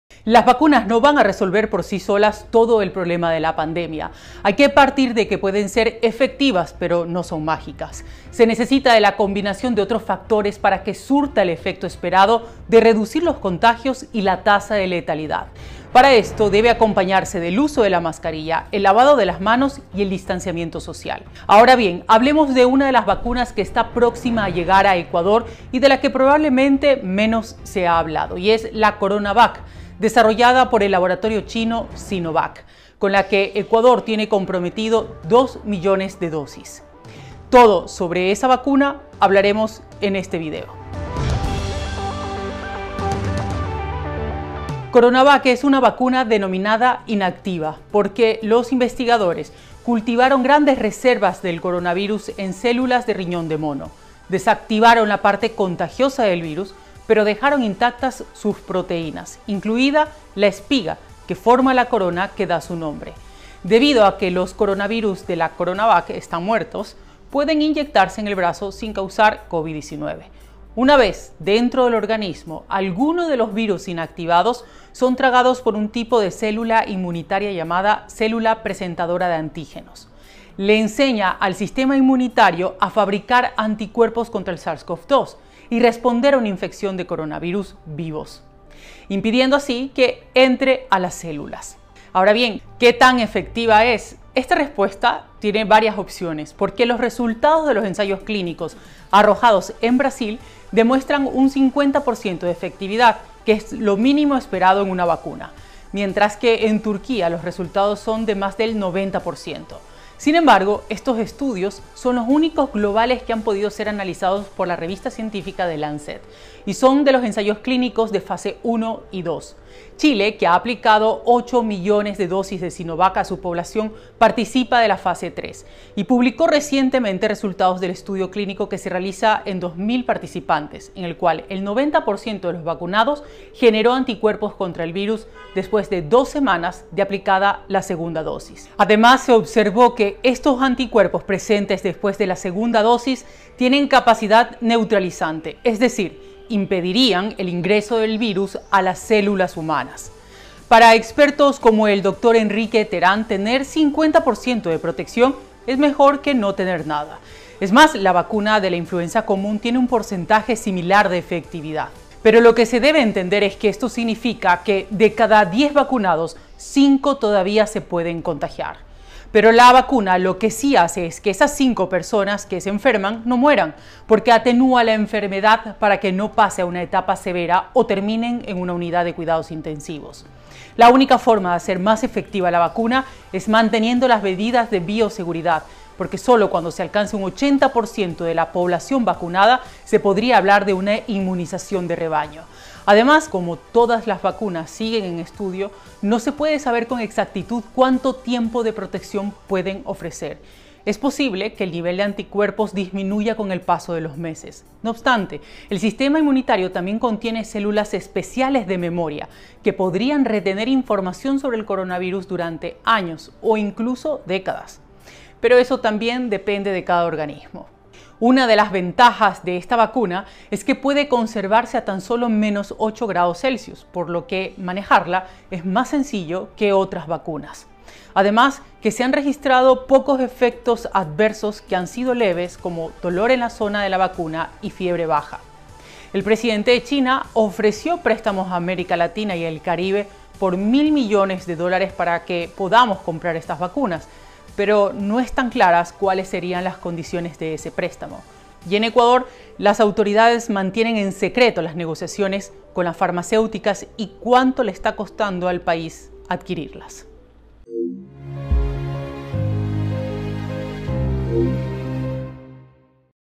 Vacuna de Sinovac es 50% efectiva, según los ensayos en Brasil
Que una vacuna de Sinovac sea 50% efectiva significa que de 10 personas 5 se podrían contagiar, pero es muy probable que no necesiten de una UCI.